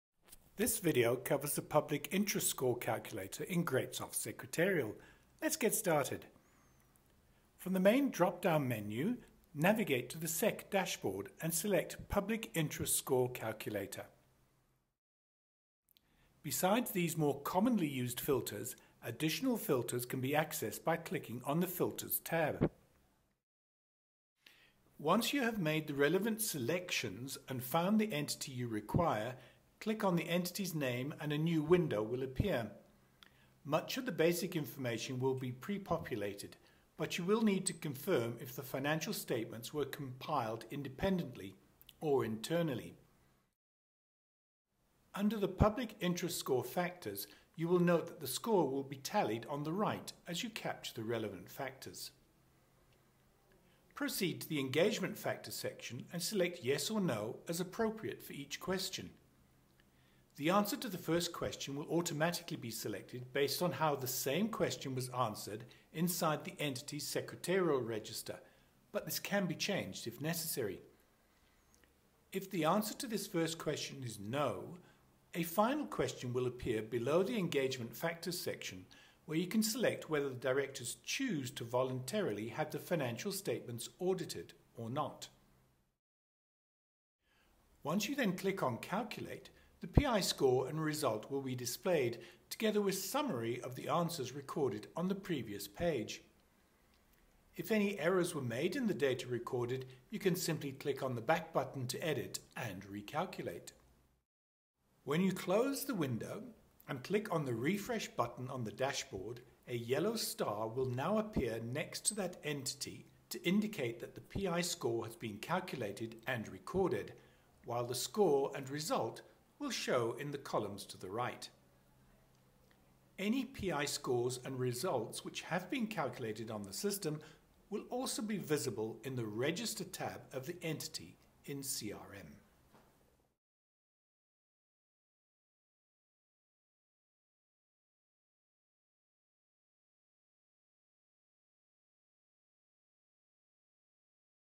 GreatSoft Secretarial - Public Interest Score Calculator
This video will guide you through the GreatSoft Public Interest Score Calculator.